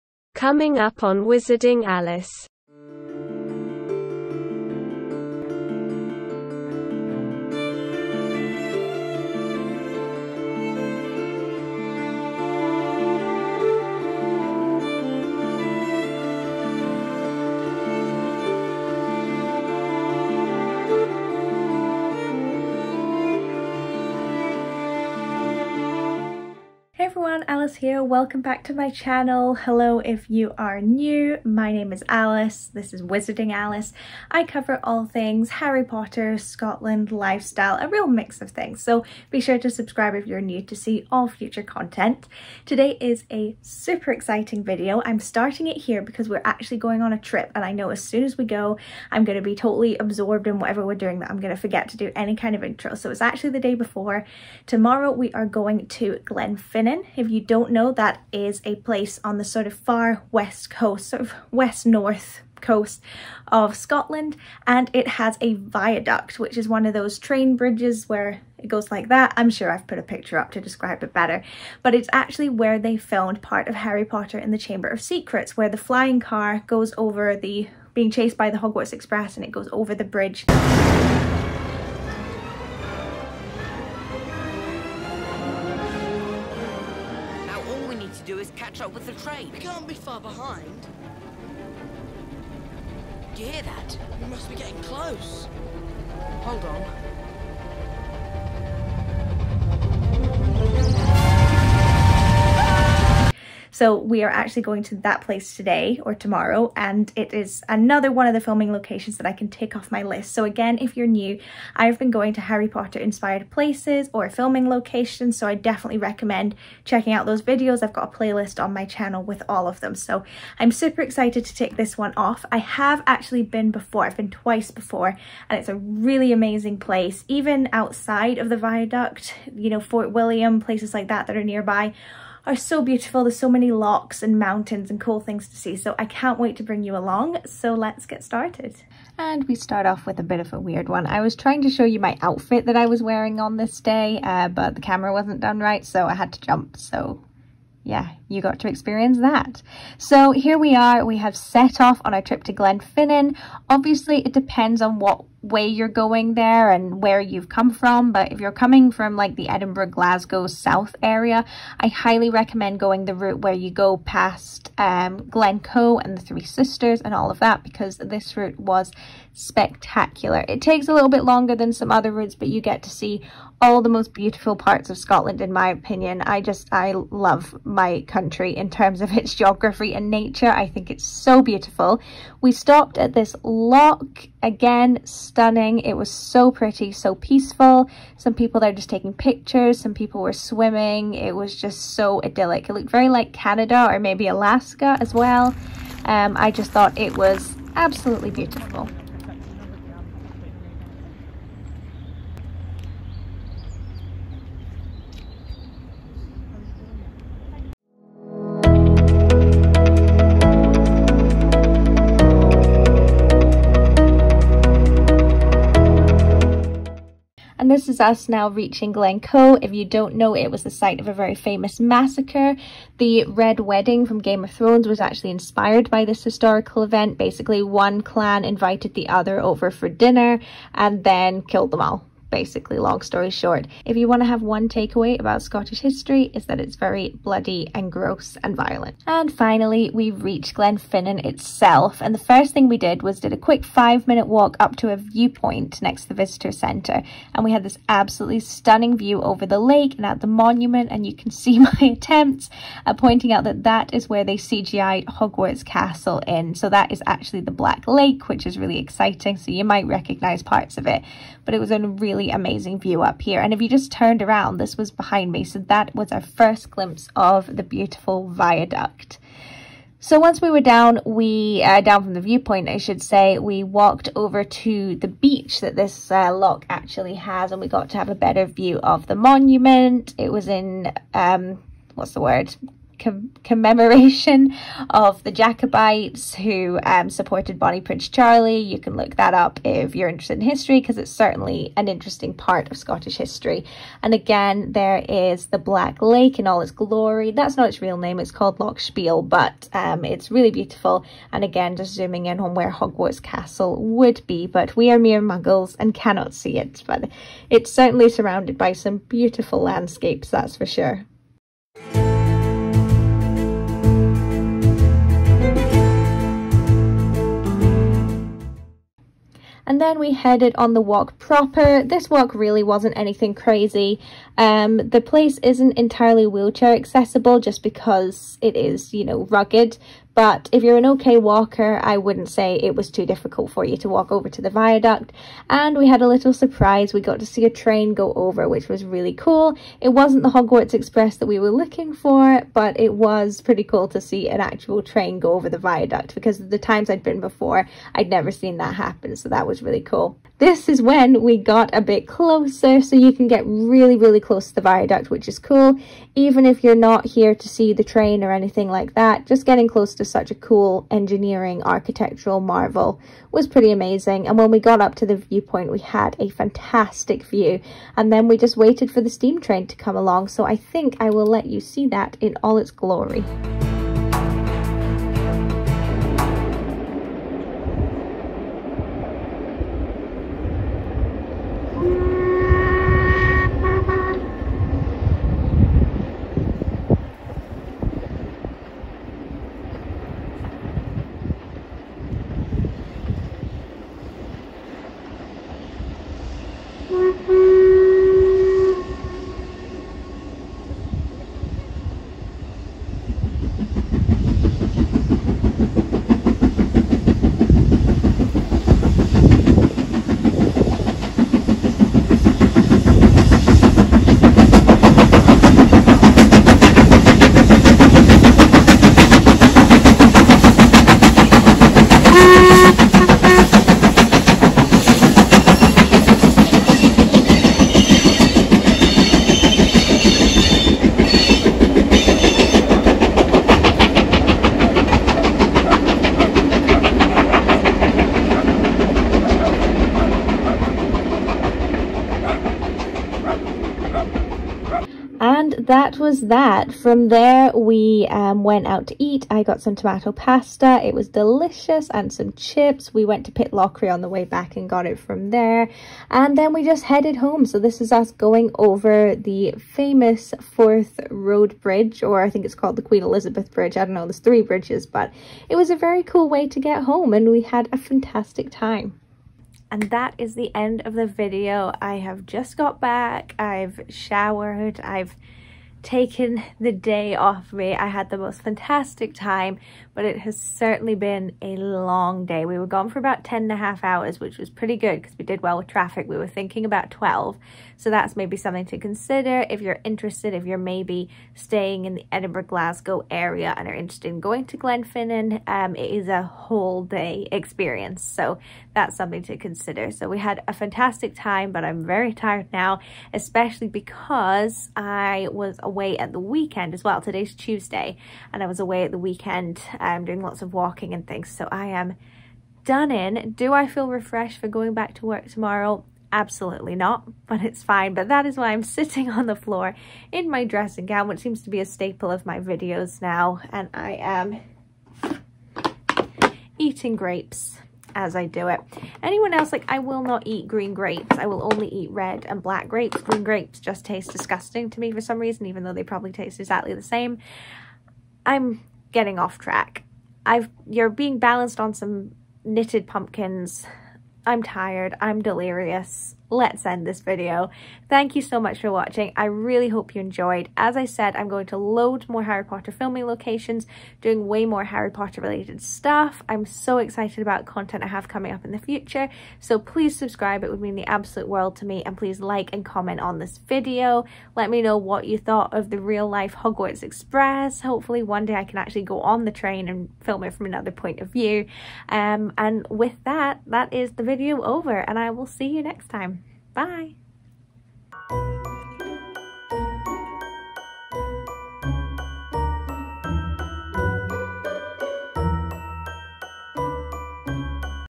Visiting The Real Life Hogwarts Express | WizardingAlice
Join me on my new adventure to Harry Potter filming locations! Today I'm visiting the Glenfinnan Viaduct, which is where they filmed some of Chamber of Secrets, as well as small parts of other films. I had so much fun, and I hope you enjoy!

If you have any ideas for things you'd like to see in future videos, please just let me know and I'd be happy to try and make that happen!